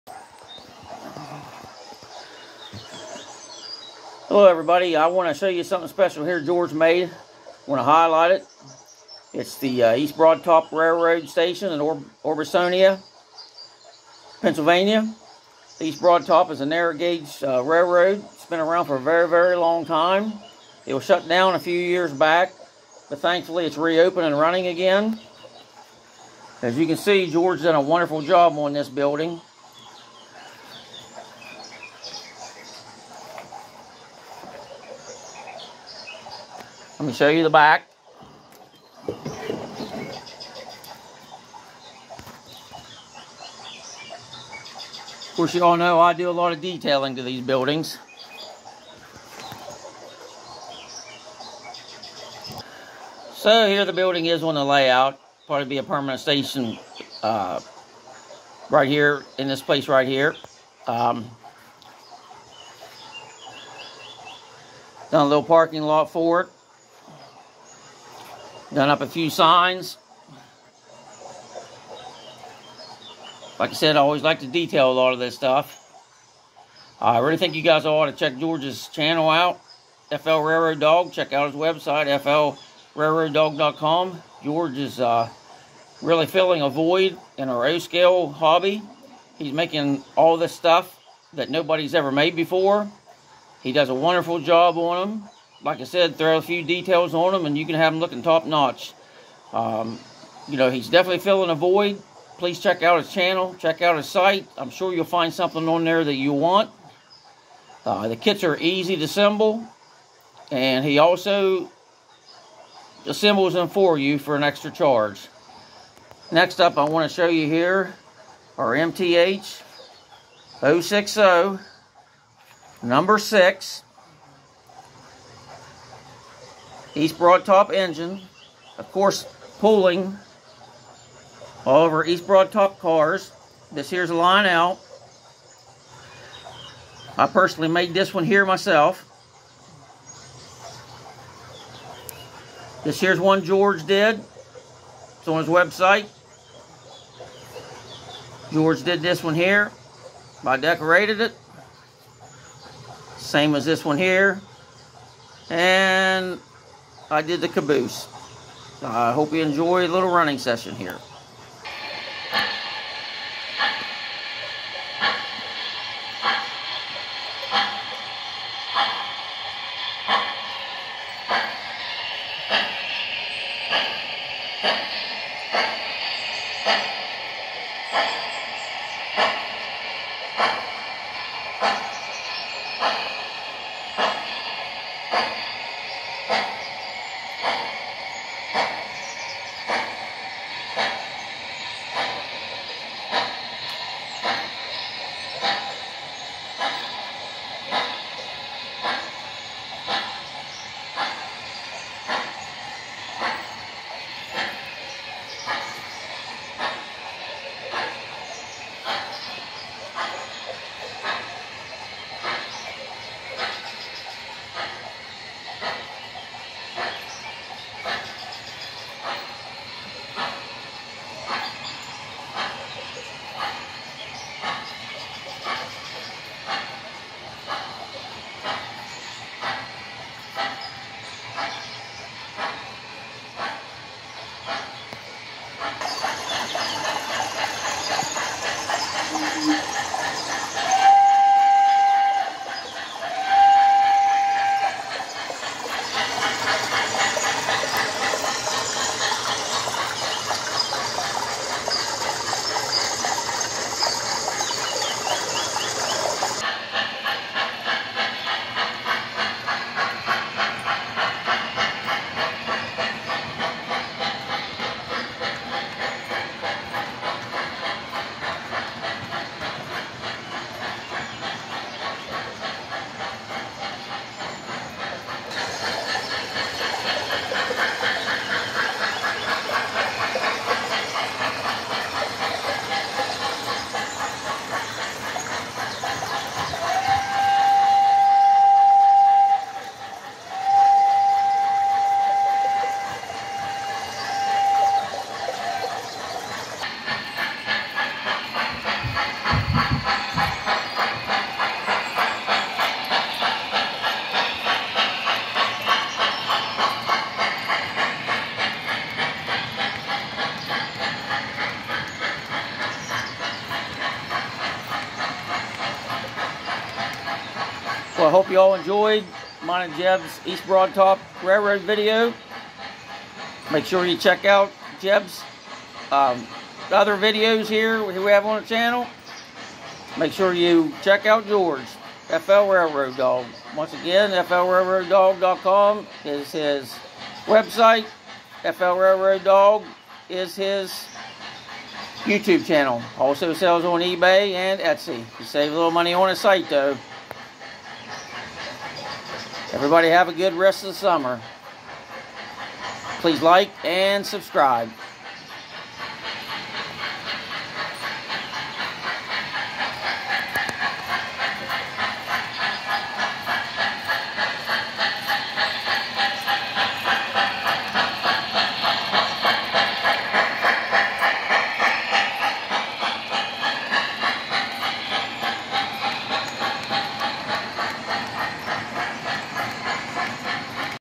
EAST BROAD TOP RAILROAD ORBISONIA STATION & MTH 0-6-0 EBT #6 in O Scale Model Railroading
This Orbisonia Station and a few of these Cars were made by Jorge Rodriguez.
ebay ,etsy, FLrailroaddog.com
They are easy to assemble Kits.  Already made in Color so NO Painting is Required, which is a huge plus.
Me and Jebb have Custom Done Up out EBT Section of the RR.  As of now there are NO EBT products made for O Scale. RMT is suppose to come out with some EBT Ore Cars.  They are not prototypical but will work. They should arrive in the near future and Sidetrack Hobbies has an 0-6-0 EBT #5 that they should be shipping soon.
When we started this EBT Project I think we were the 1st to do it in O Scale.